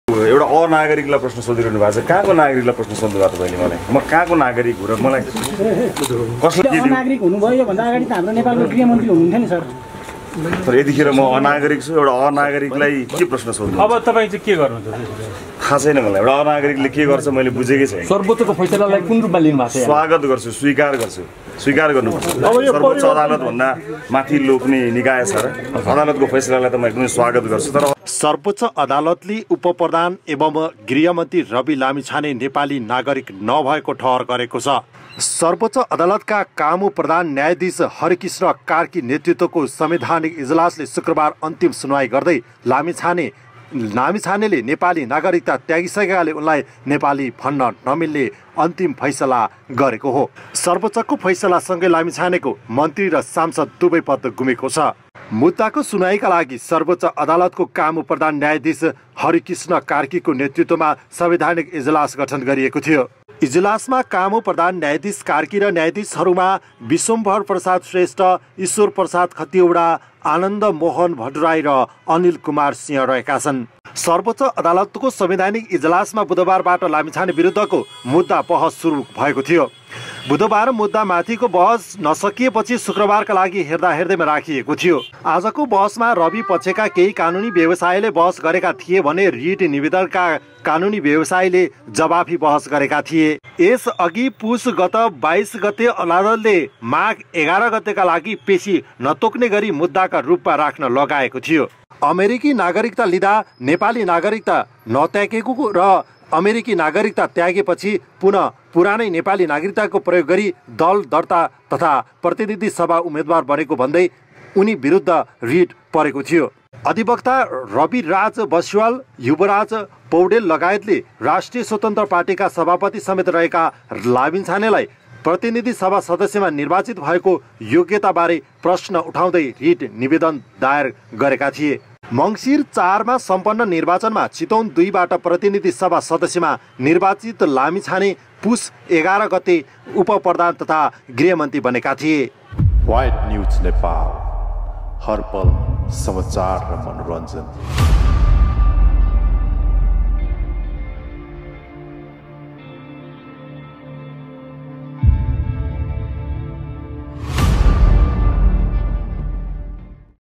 सर्बोच्चको फैसल्लापछि भावुक हुदै यसो भने रबि लामिछाने @rabi lamichhane news
White news Nepal